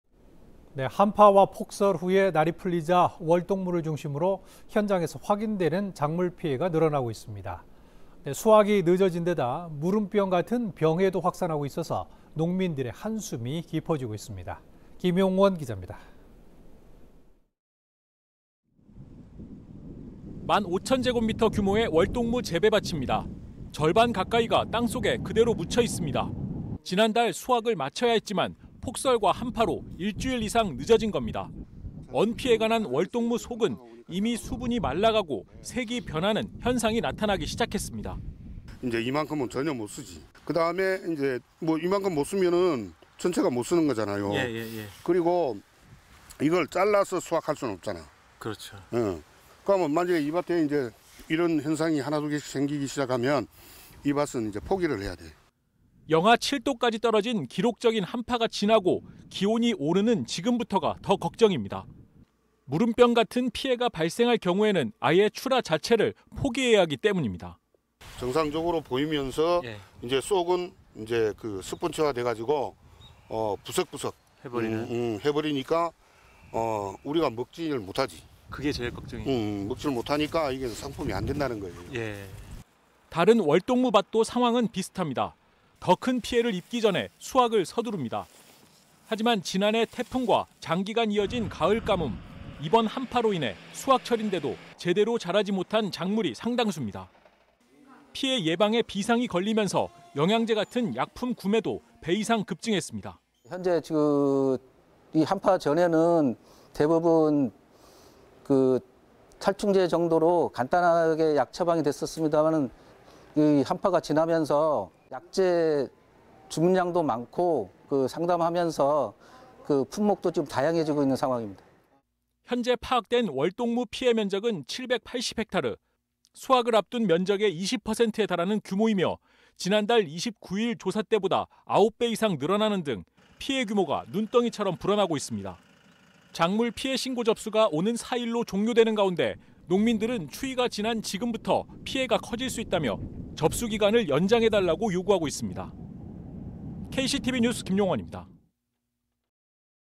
한파·폭설 지나니…월동무 피해 '눈덩이'
한파와 폭설 이후 날이 풀리면서 월동무를 중심으로 한 작물 피해도 늘어나고 있습니다.

수확도 늦어진데다 무름병 같은 병해충 피해도 확산되고 있습니다.